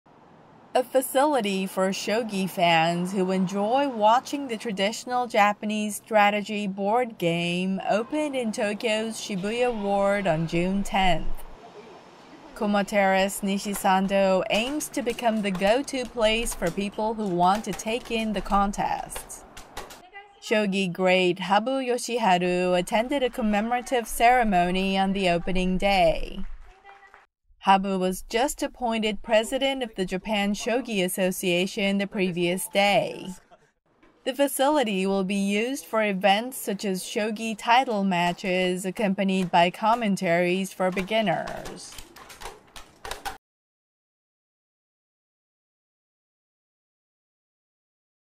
Facility for shogi spectators opens in Tokyo
A facility for shogi fans who enjoy watching the traditional Japanese strategy board game opens in Tokyo.